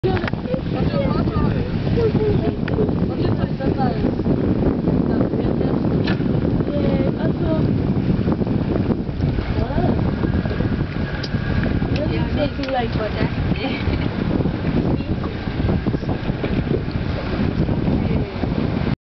Soufriere Glass Bottom Boat Ride
This is a video from a glass bottom boat ride in Soufriere, St. Lucia.